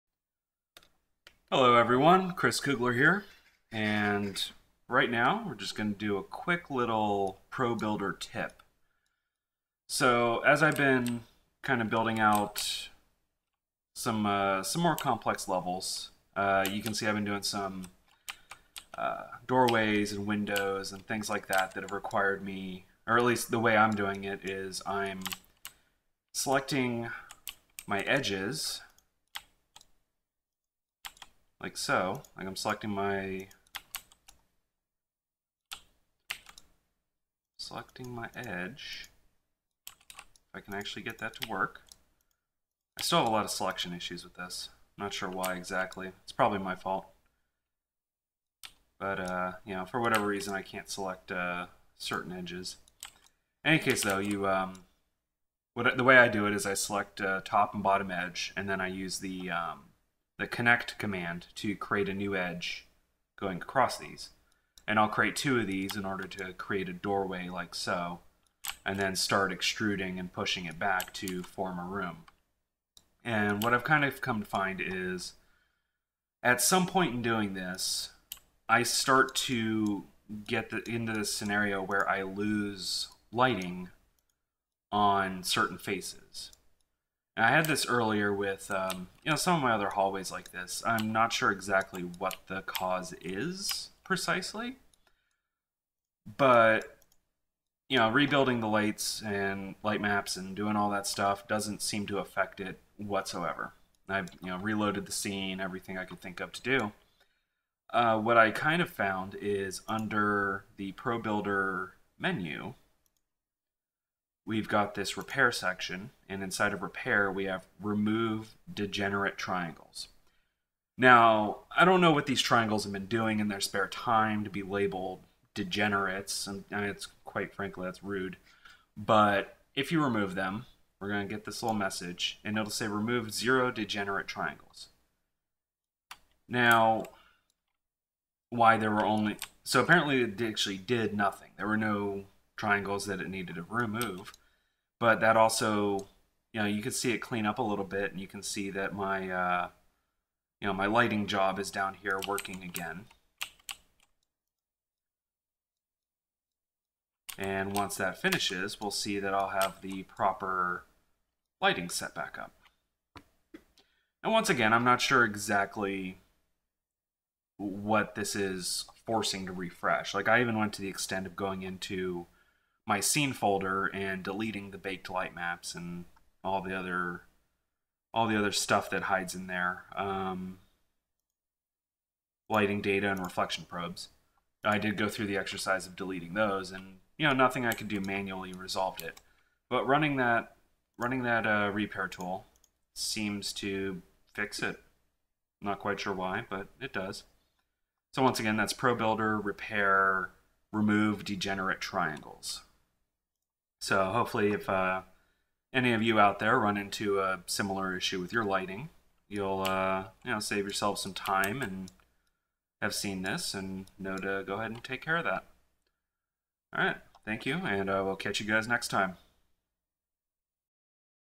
Quick Tip - Lighting Repair with ProBuilder
I came across an issue with lighting being lost when creating more complicated levels and here is a quick and easy way to force a refresh.